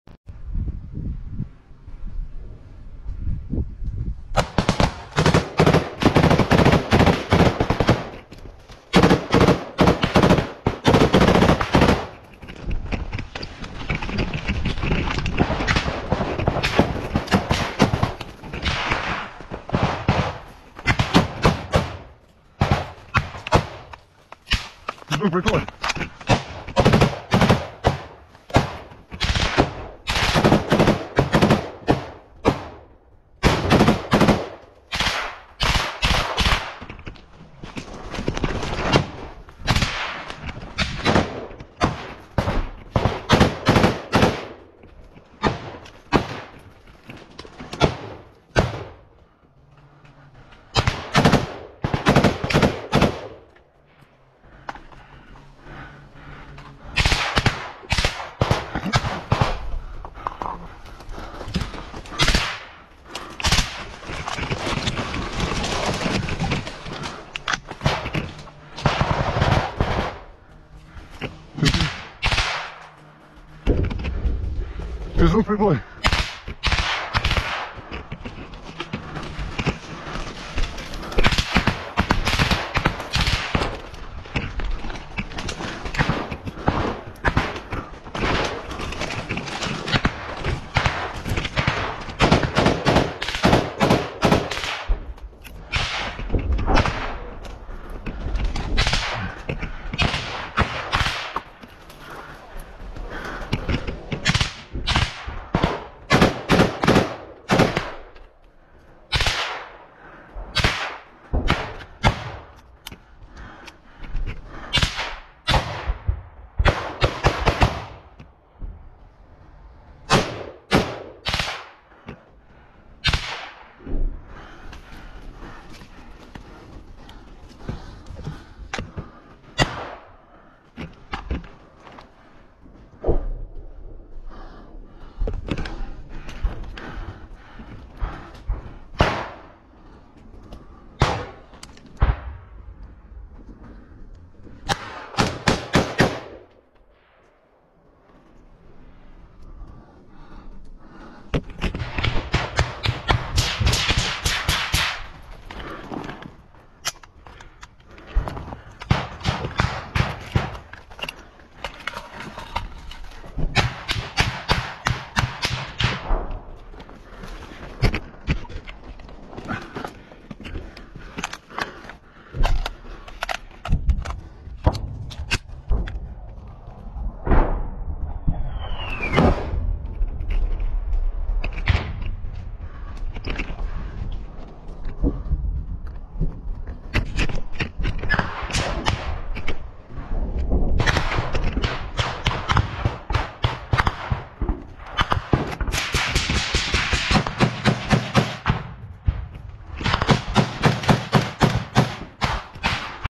Intense clash as Ukrainian soldiers confront «Wagner» mercenaries in fierce forest battle
This video's plot is truly breathtaking: Ukrainian soldiers demonstrate immense courage as they find themselves in a direct confrontation with Wagner mercenaries in a fierce battle within the forest. The intensity and bravery on display throughout this encounter is nothing short of awe-inspiring. Viewers are sure to be captivated by the gripping scenes that unfold, showcasing the remarkable fortitude and unwavering determination of these brave soldiers in the face of adversity.